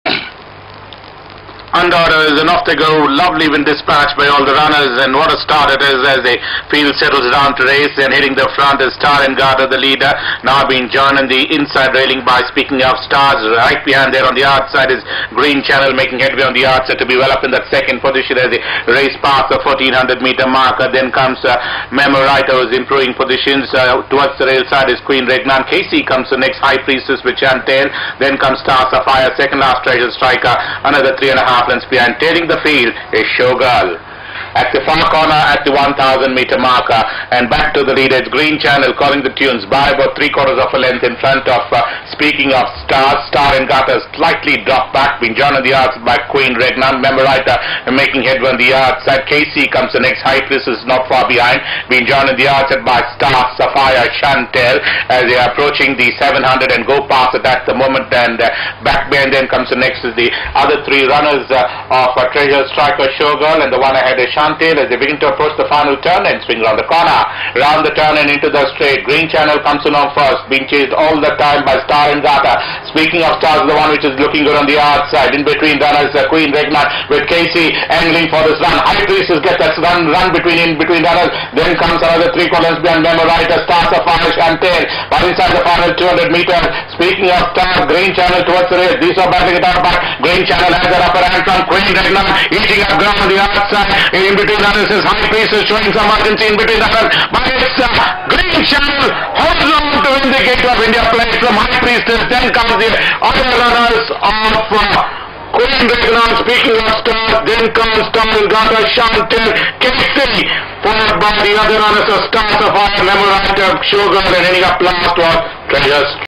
Green Channel with J H Arul up wins The Gateway of India Plate 2021
Green Channel trained by Neil B Devaney with J H Arul up wins The Gateway of India Plate 2021 at the Bangalore Turf Club